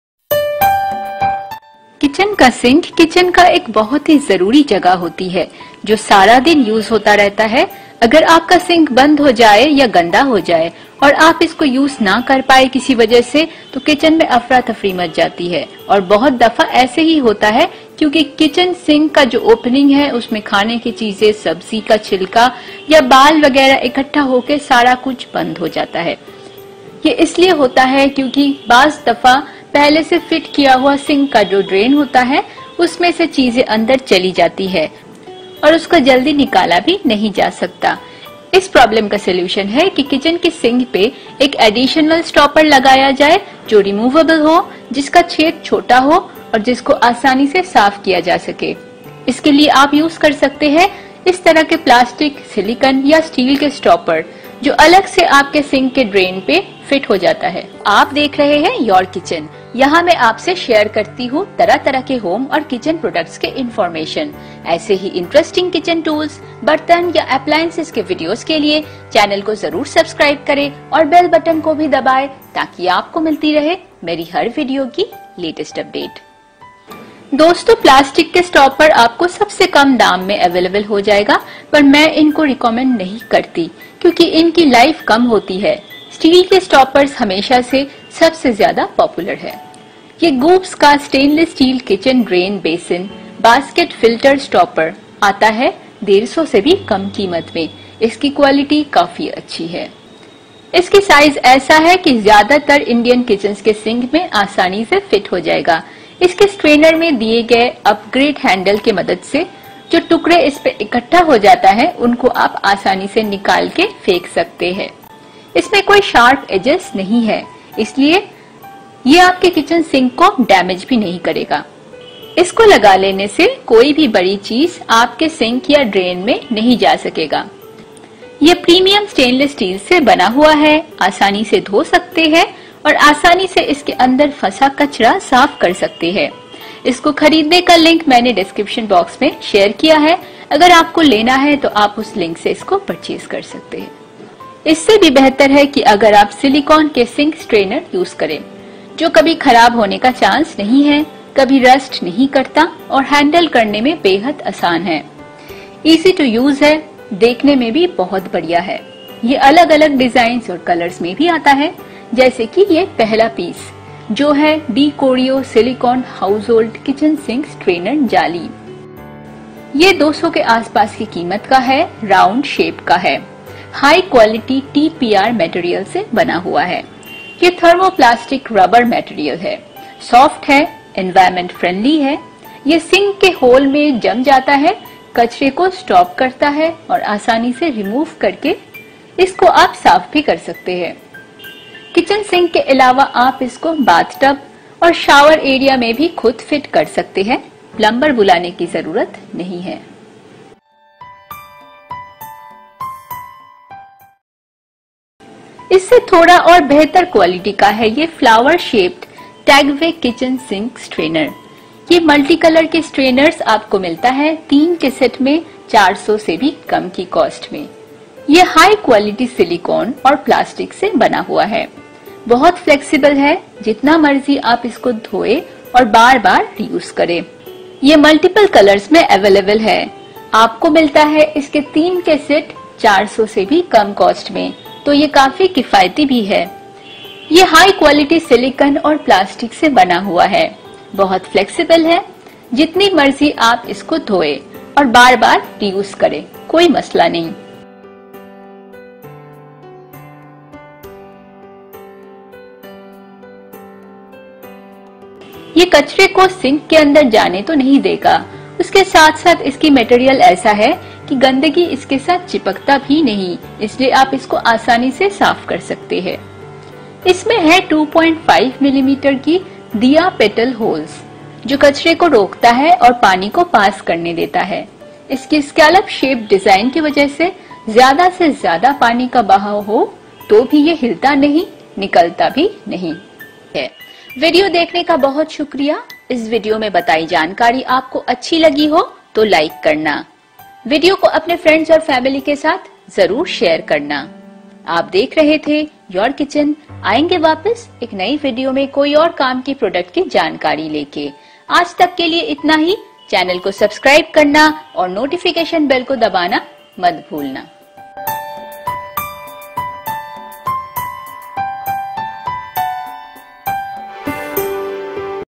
BEST SINK STRAINER || SILICONE SINK STRAINER || KITCHEN SINK STRAINER STAINLESS STEEL
watch this video on bestsinkstrainer to discover three
great options of the best sink strainers available in the market both of silicone and steel. while
siliconesinkstrainer is getting popular because of its durable, flexible and heat resistant nature, kitchensinkstrainerstainlesssteel are the all time favorite to stop the hair and tidbits from getting into your drain and clogging it.

Tagve Silicone Kitchen Sink Strainer Flower Shaped, Set of 3

DeCorio Silicone Household Kitchen Sink Strainer

Goopz Stainless Steel Kitchen Drain Filter Stopper (Medium)